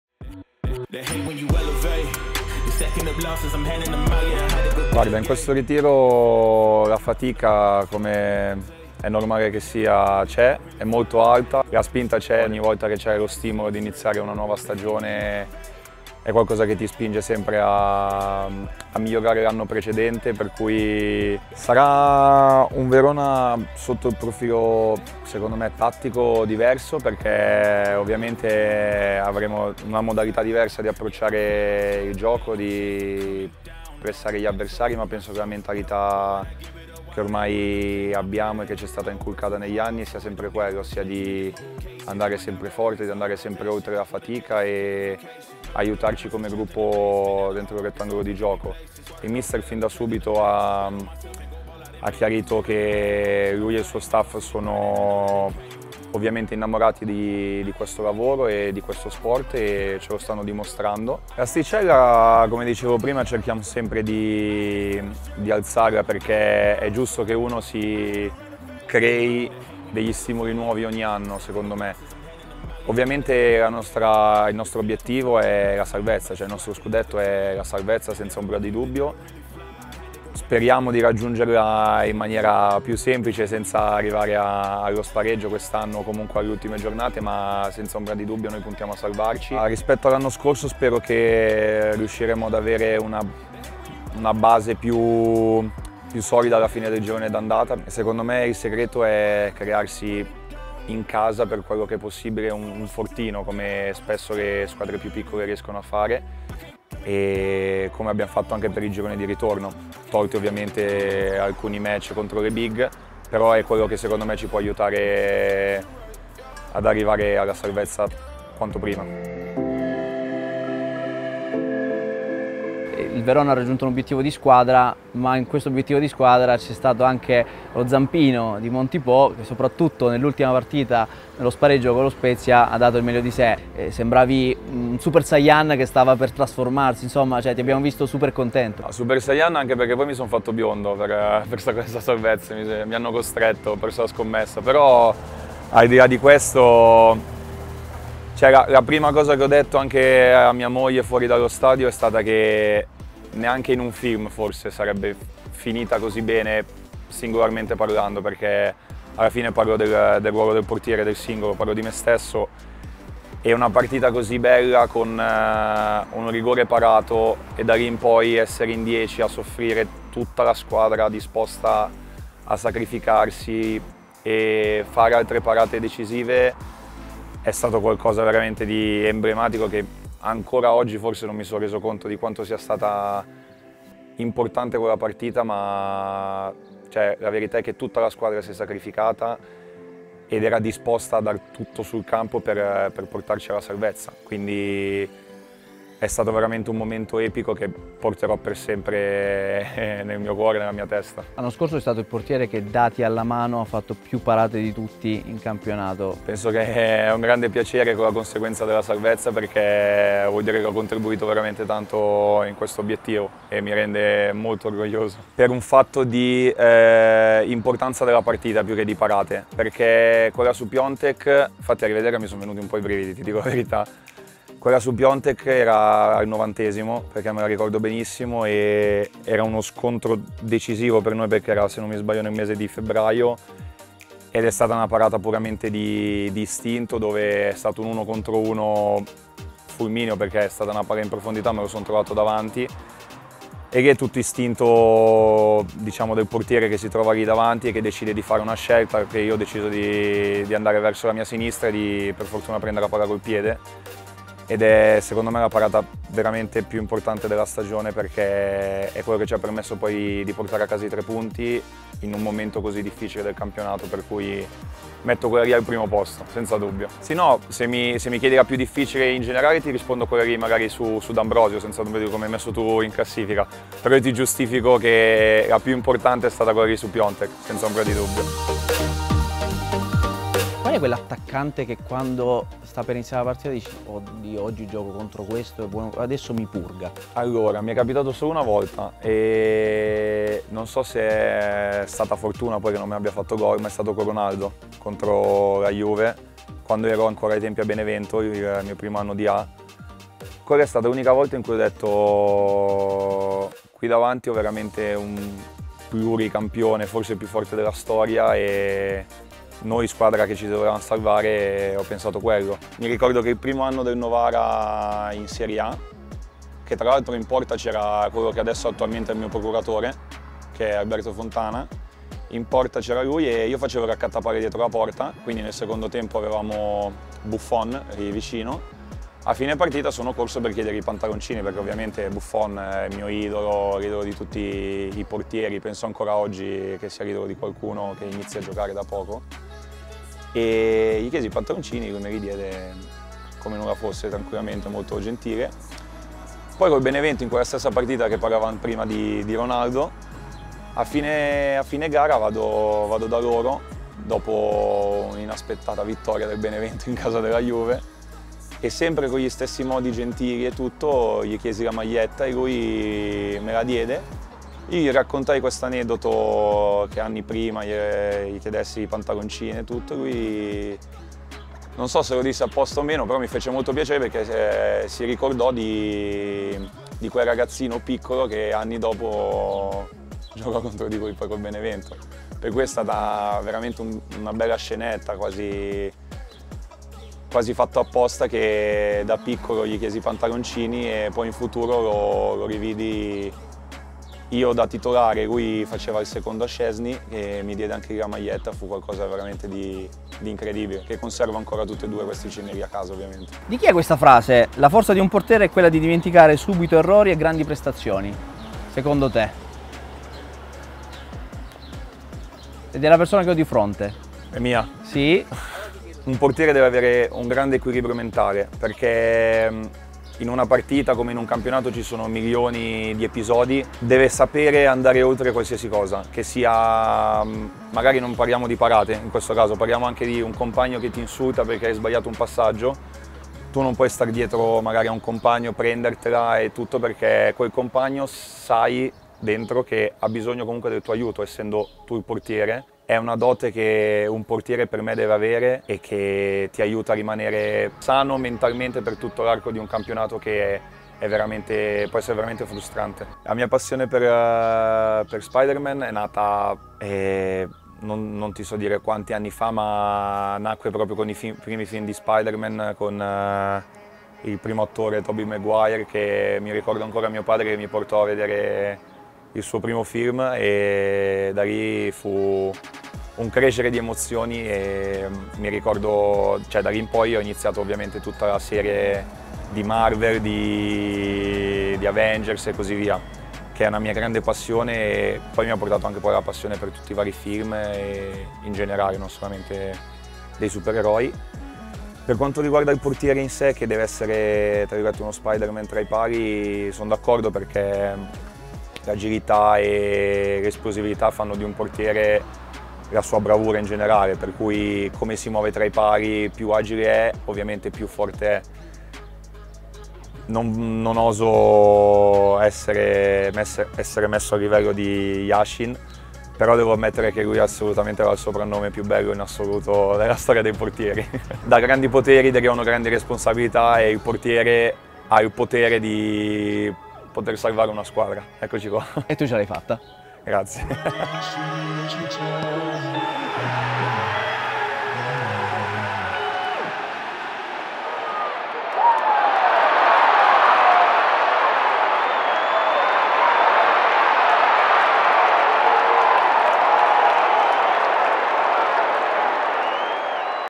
Pre-Season Tour | Discover Verona with Lorenzo Montipò | Serie A 2023/24
Verona's long and complicated season was decided by the play-off against Spezia that earned them a place in Serie A. Lorenzo Montipò took centre stage in the last campaign, with the Gialloblu goalkeeper putting on his superhero cape in the decisive showdown against the Eagles | Serie A 2023/24